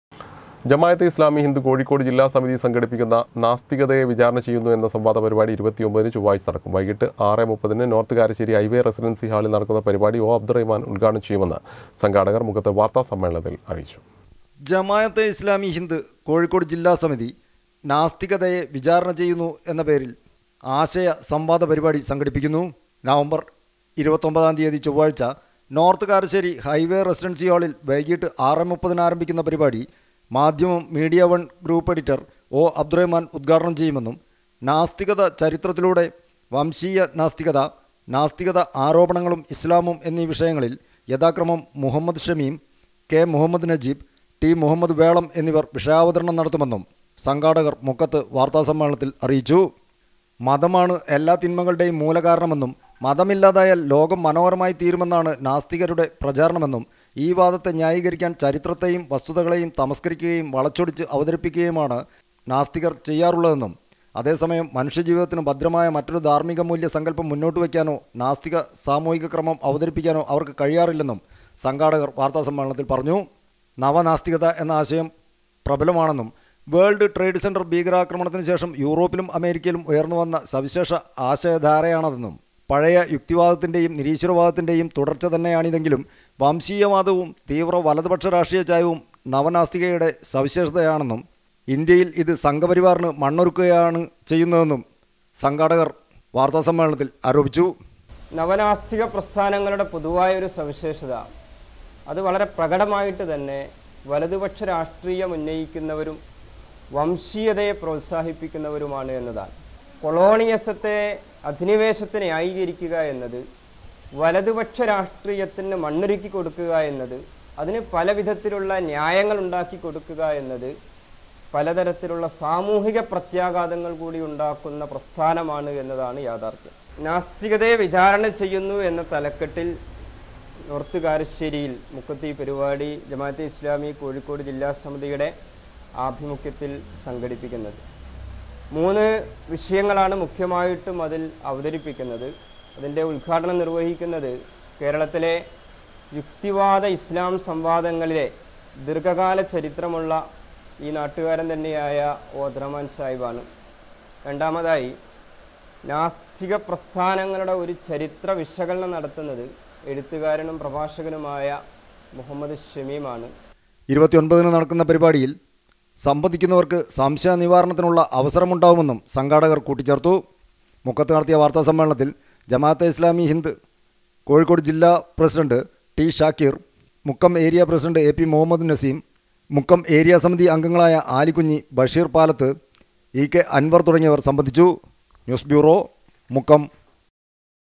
ജമാ അതെ ഇസ്‌ലാമി ഹിന്ദ് കോഴിക്കോട് ജില്ലാ സമിതി സംഘടിപ്പിക്കുന്ന നാസ്തികതയെ വിചാരണ ചെയ്യുന്നു
ജമാ അതെ ഇസ്‌ലാമി ഹിന്ദ് കോഴിക്കോട് ജില്ലാ സമിതി സംഘടിപ്പിക്കുന്ന  നാസ്തികതയെ വിചാരണ ചെയ്യുന്നു എന്ന സംവാദ പരിപാടി 29  നു ചൊവ്വാഴ്ച നടക്കും. വൈകീട്ടും 6 . 30 നു നോർത്ത് കാരശ്ശേരി ഹൈവേ റെസിഡൻസി ഹാളിൽ നടക്കുന്ന പരിപാടി ഓ.അബ്ദുറഹിമാൻ ഉത്ഘാടനം ചെയ്യുമെന്ന്